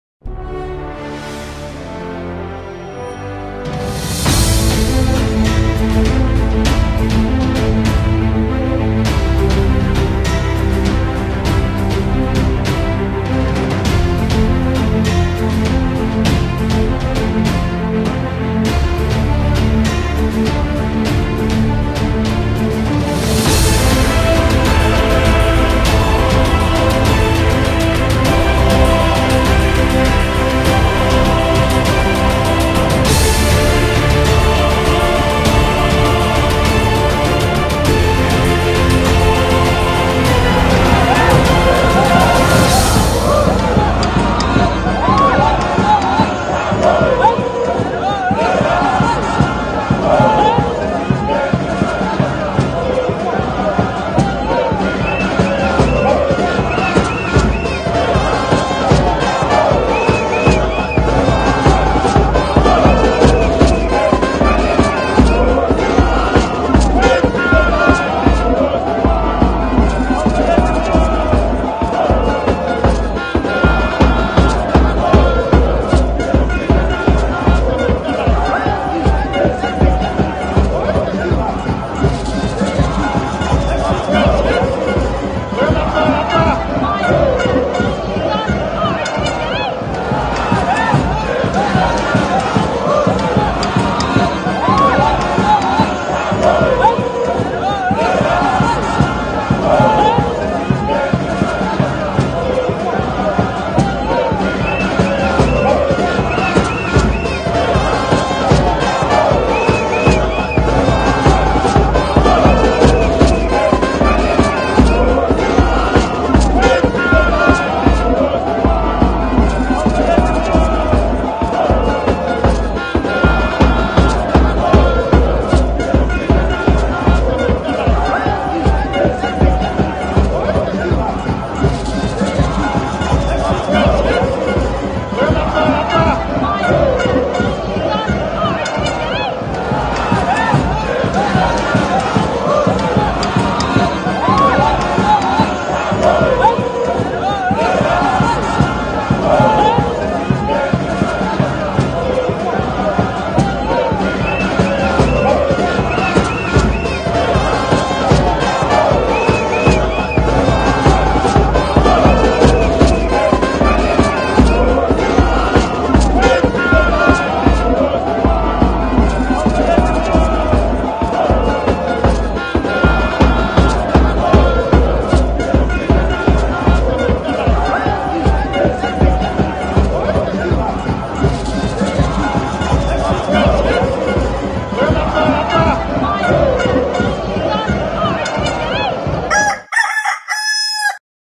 GHANAIANS JUBILATING AFTER A 3-2 WIN AGAINST SOUTH KOREA
Breaking Political News GHANAIANS JUBILATING AFTER A 3-2 WIN AGAINST SOUTH KOREA